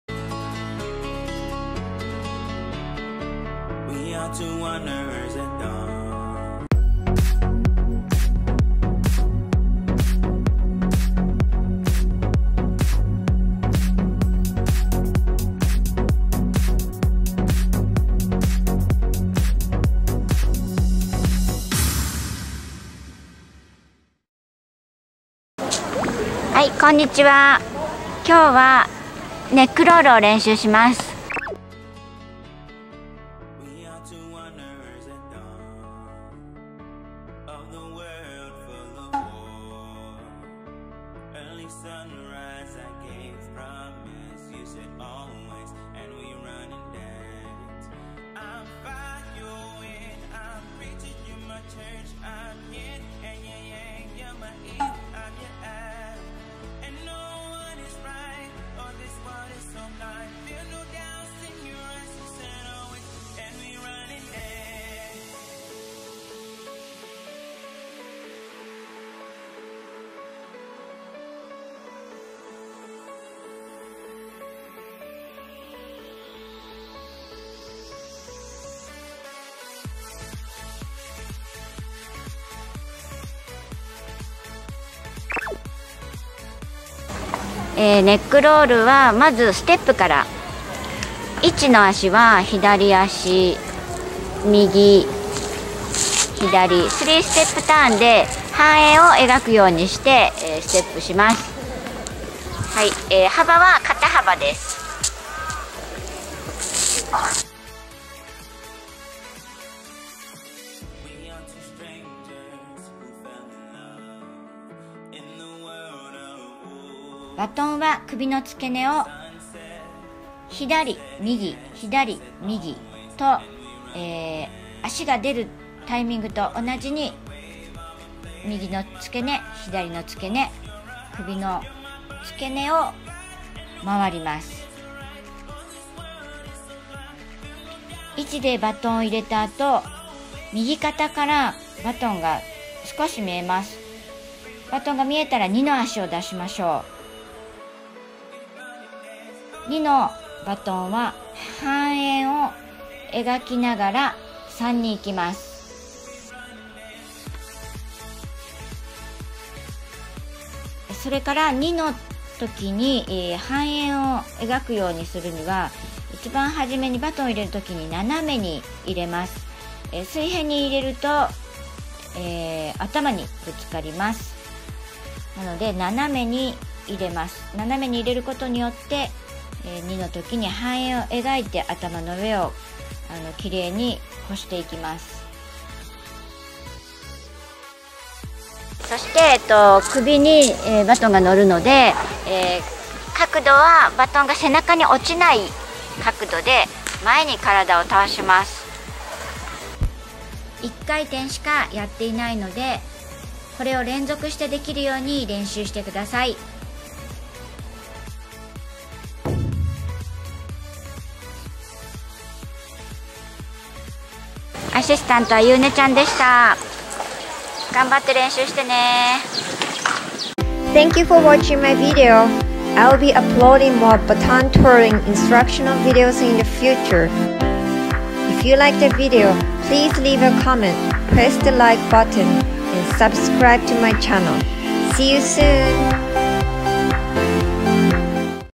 YUKA BATON LESSON NECK ROLL TECHNIQUE VIDEO
We will show the lesson of neck roll technique video.
Do you enjoy my tutorial videos?

I continue the "Baton Twirling lesson " series from the "Novice Baton Twirling" to " Expert Baton Twirling " segment by teaching the some technique.
This move is great for new twirlers because it starts working on basic hand eye coordination and wrist flexibility with twirling. This twirl is also the starting point to learning more difficult moves in the future.  I go over this trick step by step making sure to go over proper body work and technique.
Also, feel free to post a video response of you doing this trick! I would love to see your progress!